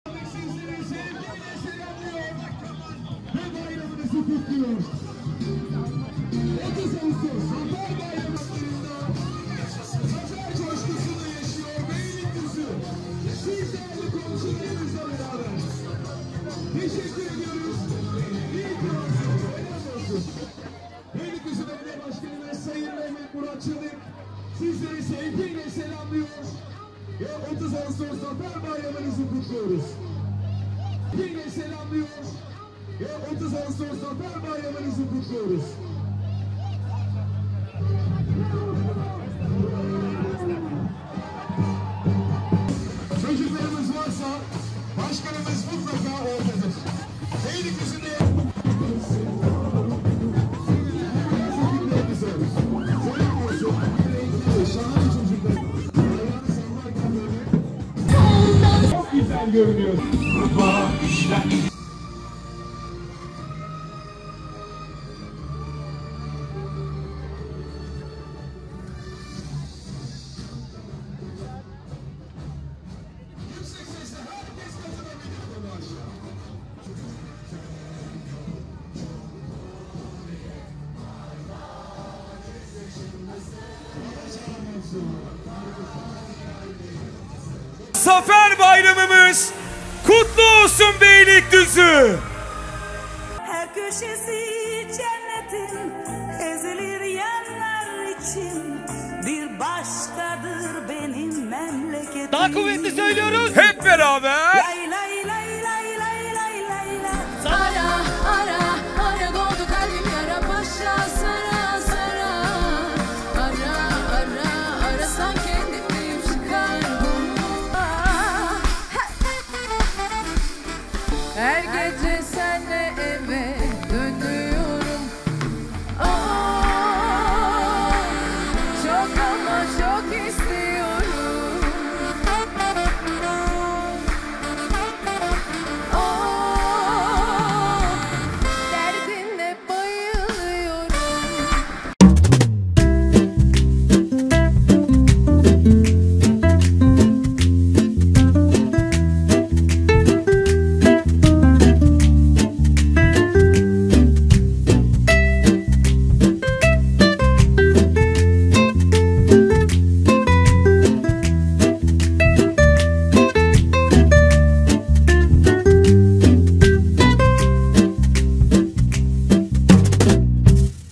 BEYLİKDÜZÜ 30 AĞUSTOS ZAFER BAYRAMI
Beylikdüzü Barış ve Sevgi Buluşmaları Etkinliklerinde 30 Ağustos Zafer Bayramı dolası ile kortej ve konser görüntüleri